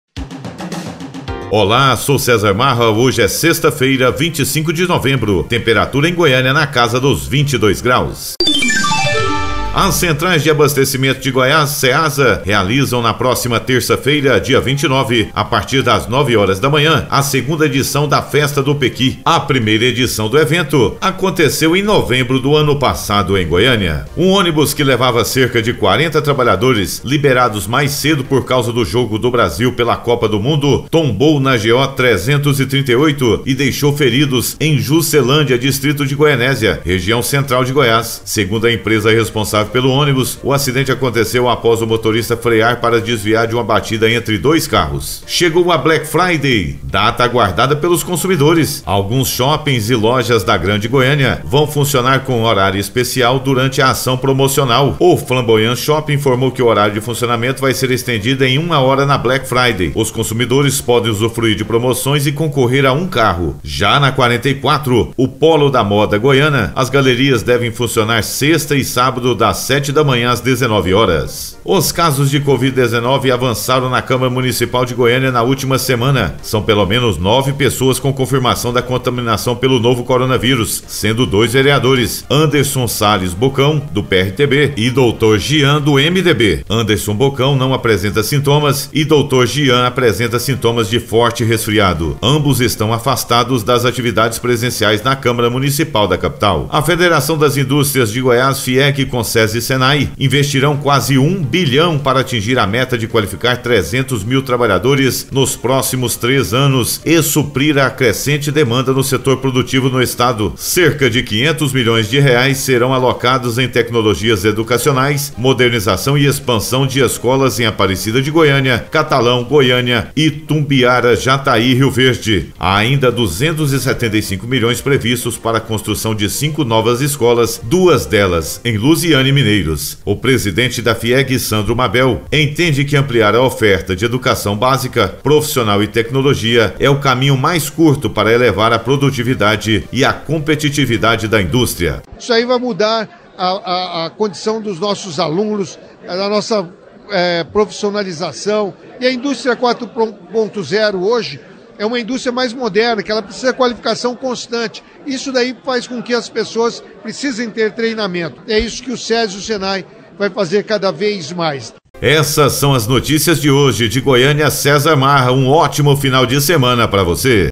Noticias de Goiás - Correspondente César Marra (25/11/2022)
As Centrais de Abastecimento de Goiás (Ceasa Goiás) realizam, na próxima  terça-feira (29), a partir das 9h, a segunda edição da Festa do Pequi. A primeira edição do evento aconteceu em novembro do ano passado  em Goiânia.

Um ônibus que levava cerca de 40 trabalhadores liberados mais cedo por causa do jogo do Brasil pela Copa do Mundo tombou na GO-338 e deixou feridos, em Juscelândia, distrito de Goianésia,  região central de Goiás.
Segundo a empresa responsável pelo ônibus, o acidente aconteceu após o motorista frear para desviar de uma batida entre dois carros.

Chegou a BlackFriday, data aguardada pelos consumidores , Alguns shoppings e lojas da Grande Goiânia vão funcionar com horário especial durante a ação promocional.
O Flamboyant Shopping informou que o horário de funcionamento vai ser estendido em uma hora na Black Friday. Os consumidores podem usufruir de promoções e concorrer a um carro.
ja na região do polo da 44 As galerias devem funcionar  Sexta-feira e sabado das 7h às 19h .

Os casos de Covid-19 avançaram na Câmara Municipal de Goiânia na última semana. São pelo menos 9 pessoas com confirmação de contaminação pelo coronavírus  sendo 2 vereadores , Anderson Sales Bokão (PRTB) e Dr Gian (MDB), que é médico.

Anderson   Bokão não apresenta sintomas, e doutor Gian apresenta sintomas de “forte resfriado”. Ambos estão afastados das atividades presenciais na Câmara Municipal da capital goiana.

A Federação das Indústrias de Goiás (FIEG), com o Sesi e o Senai, investirão quase R$ 1 bilhão para atingir a meta de qualificar 300 mil trabalhadores nos próximos três anos e suprir a crescente demanda do setor produtivo no estado. Cerca de 500 milhões de reais serão alocados em tecnologias educacionais, modernização e expansão de escolas em Aparecida de Goiânia, Catalão, Goiânia, Itumbiara, Jataí e Rio Verde. Há ainda R$ 275 milhões previstos para construção de cinco novas escolas, duas delas em Luziânia e Mineiros.

O presidente da FIEG, Sandro Mabel, entende que ampliar a oferta de educação básica, profissional e tecnológica é o caminho mais curto para elevar a produtividade e a competitividade da indústria.

Fonte: Brasil 61